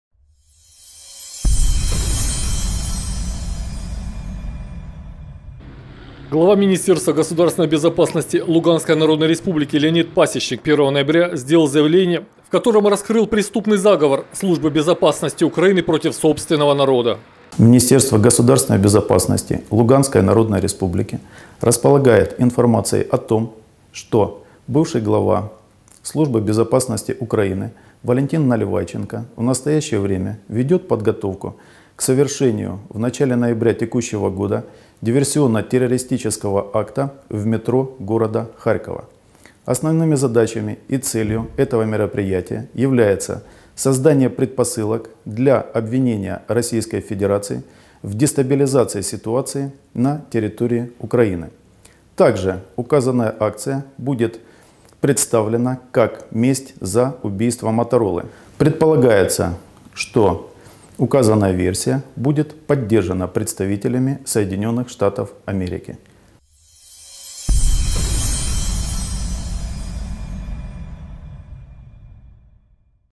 Заявление главы МГБ ЛНР
Глава МГБ ЛНР Леонид Пасечник 1 ноября сделал заявление, в котором раскрыл преступный заговор СБУ Украины против собственного народа.